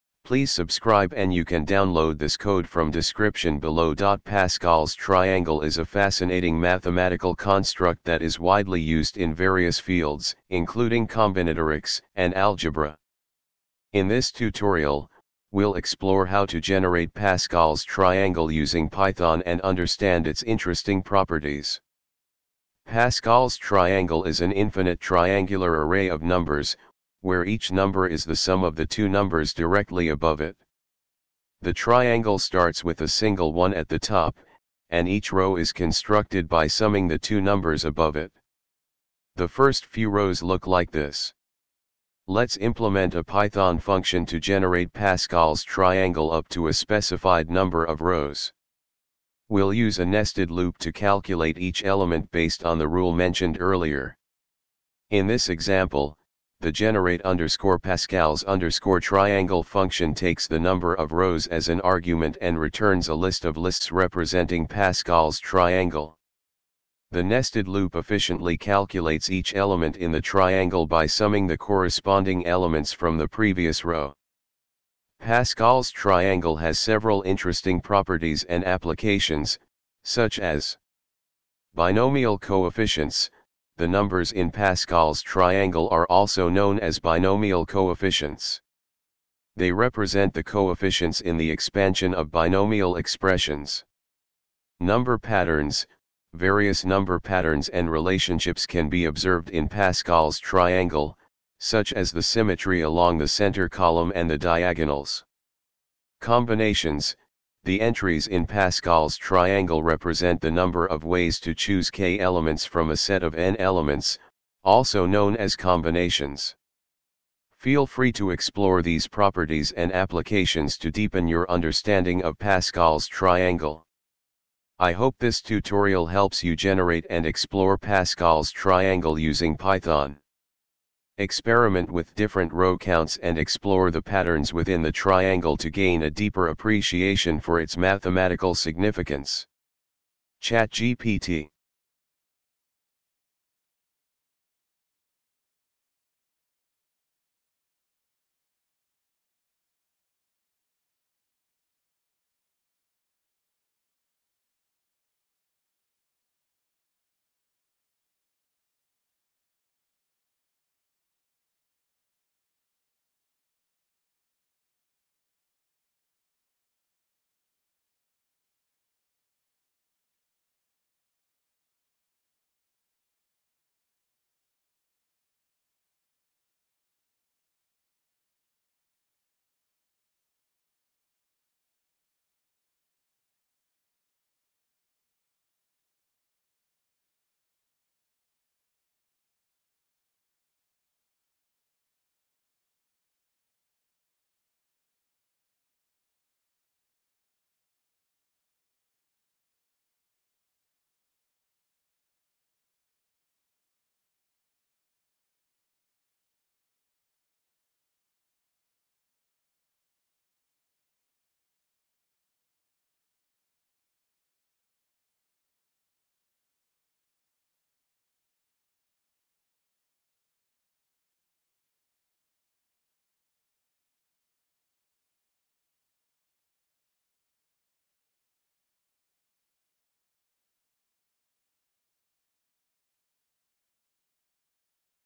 Pascal s Triangle Python
Pascal's Triangle is a fascinating mathematical construct that is widely used in various fields, including combinatorics and algebra. In this tutorial, we'll explore how to generate Pascal's Triangle using Python and understand its interesting properties.
Pascal's Triangle is an infinite triangular array of numbers, where each number is the sum of the two numbers directly above it. The triangle starts with a single "1" at the top, and each row is constructed by summing the two numbers above it. The first few rows look like this:
Let's implement a Python function to generate Pascal's Triangle up to a specified number of rows. We'll use a nested loop to calculate each element based on the rule mentioned earlier.
In this example, the generate_pascals_triangle function takes the number of rows as an argument and returns a list of lists representing Pascal's Triangle. The nested loop efficiently calculates each element in the triangle by summing the corresponding elements from the previous row.
Pascal's Triangle has several interesting properties and applications, such as:
Binomial Coefficients: The numbers in Pascal's Triangle are also known as binomial coefficients. They represent the coefficients in the expansion of binomial expressions.
Number Patterns: Various number patterns and relationships can be observed in Pascal's Triangle, such as the symmetry along the center column and the diagonals.
Combinations: The entries in Pascal's Triangle represent the number of ways to choose k elements from a set of n elements, also known as combinations.
Feel free to explore these properties and applications to deepen your understanding of Pascal's Triangle.
I hope this tutorial helps you generate and explore Pascal's Triangle using Python. Experiment with different row counts and explore the patterns within the triangle to gain a deeper appreciation for its mathematical significance.
ChatGPT